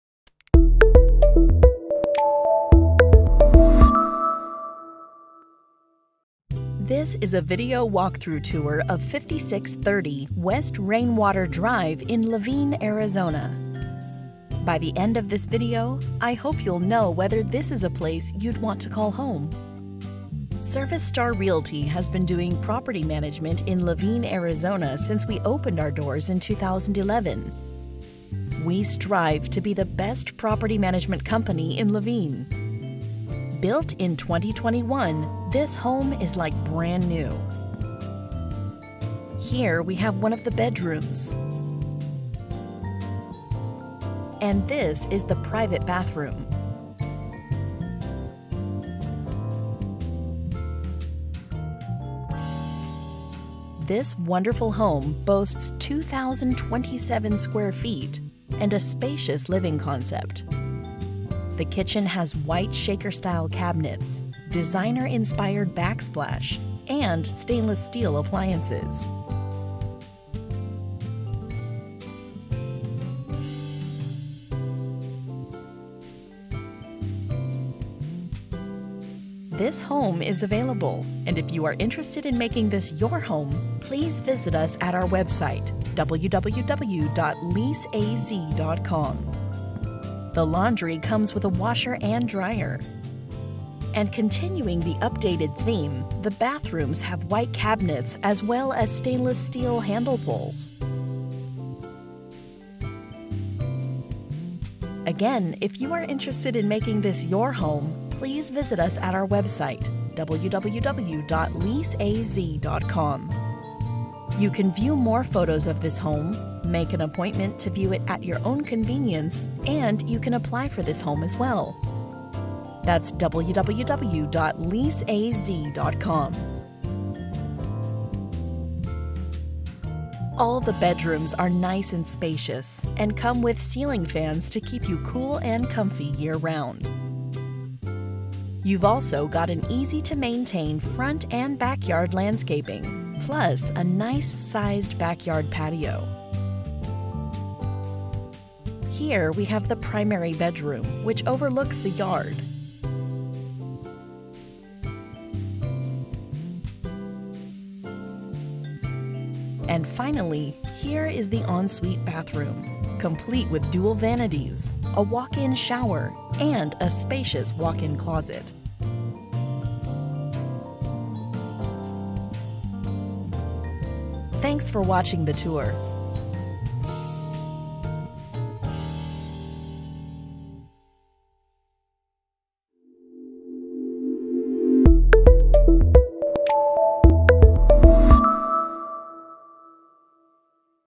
Laveen Homes for Rent 4BR/3BA by Laveen Property Management | Service Star Realty
Built in 2021, this home is like brand new. Boasting 2027 square feet and a spacious living concept. The kitchen features white Shaker style cabinetry, designer inspired backsplash, center island, stainless steel appliances, recessed lighting, granite-like countertops, gas cooktop, and large pantry. Continuing the updated theme throughout this almost new home, bathrooms have white cabinets, stainless steel handle pulls, and beautiful tiled shower. Easy to maintain front and backyard landscaping and nice sized backyard patio.

Looking for a property manager in Laveen? We're the specialists in Laveen property management!

Let us manage your Laveen Rental Property